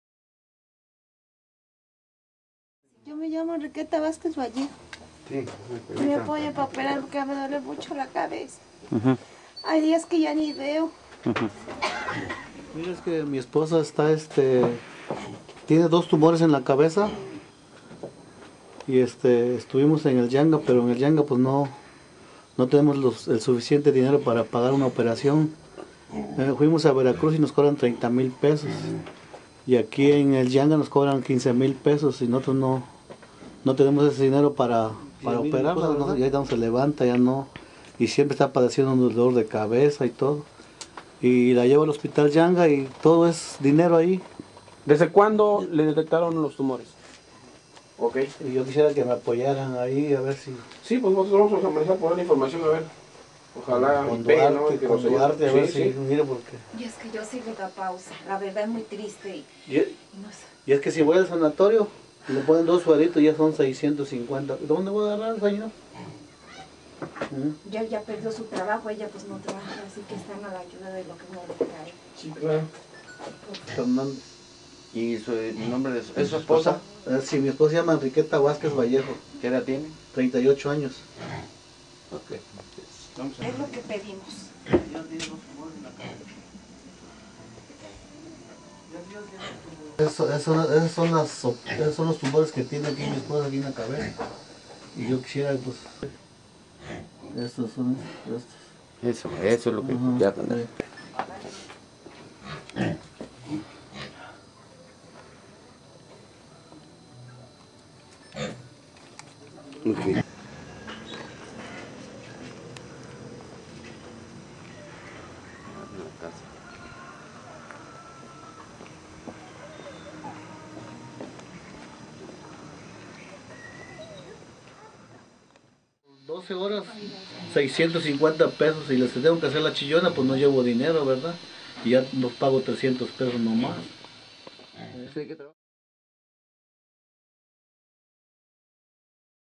Ama de casa pide ayuda a las autoridades.
Desde hace más de un mes la señora Enriqueta Vázquez Vallejo, de 38 años de edad, se encuentra en una cama sin poder pararse, pues debido a este grave problema no tiene  fuerza y en ocasiones pierde la vista; tiene dos hijas menores de edad de 13 y 14 años, y además su esposo se ha quedado sin trabajo para poder ayudar en la economía familiar.
El licenciado Francisco Portilla me visitó en el hospital y dijo que me apoyaría pero me engañaron, no recibí ningún apoyo, no tenemos trabajo, espero que el señor Gobernador pueda ayudarme, los dolores son intensos, debido a que en caso de que no sea atendida, podría morir lentamente.